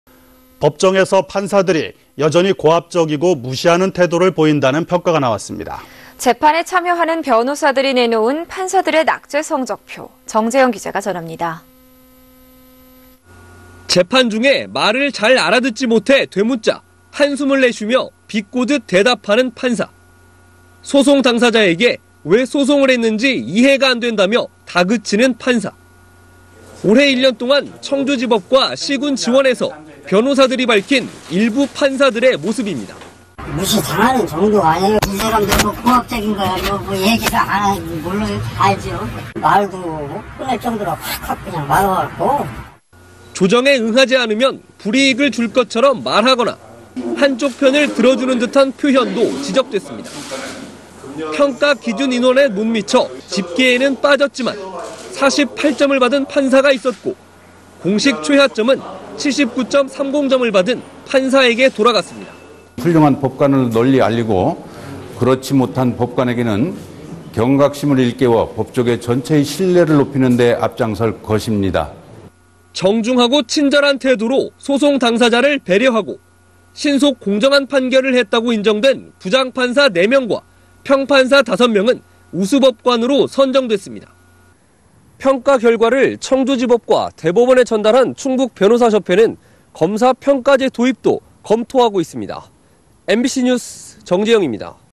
[청주MBC뉴스] 고압·무시 여전한 법원
◀앵커▶
 도내 변호사들이 재판에서 만나는 판사들을
평가해 성적표를 내놨습니다. 많이 나아졌다는 데 일부 판사들의 고압적이고 무시하는듯한
태도는 여전했습니다. 정재영 기자입니다.
           ◀ＥＮＤ▶

           ◀ＶＣＲ▶
CG1
 재판 중에 말을 잘 알아듣지 못해 되묻자
한숨을 내쉬며 비꼬듯 대답하는 판사.

CG2
 소송당사자에게 왜 소송했는지
이해가 안된다며 다그치는 판사.

 올해 1년동안 청주지법과 시·군 지원에서
변호사들이 밝힌 일부 판사들의 모습입니다.

    ◀ＳＹＮ▶청주지법 재판 경험자
"무시당하는 정도가 아니에요. 이 사람들
고압적인 거야 얘기 안 하면 몰라요? 다 알죠.
말도 못 꺼낼 정도로 확확 그냥 막아버리고."

 조정에 응하지 않으면 불이익을 줄 것처럼
말하거나, 한쪽 편을 들어주는 듯한 표현도
지적됐습니다.

 평가기준인원에 못미쳐 집계에는 빠졌지만
48점을 받은 판사가 있었고  공식 최하점은 79.30점을 받은 판사에게 돌아갔습니다.

◀ＩＮＴ▶김교형 변호사/법관평가특위 위원장
"훌륭 법관 알리고 아닌 법관 경각심 일깨울것"

(C.G) 정중하고 친절한 태도로 소송당사자를
배려하고 신속·공정한 판결을 했다고 인정된
부장판사 4명과 평판사 5명은 우수법관으로

(S/U)평가 결과를 청주지법과 대법원에
전달한 충북변호사협회는 검사평가제 도입도
검토하고 있습니다. MBc뉴스 정재영입니다.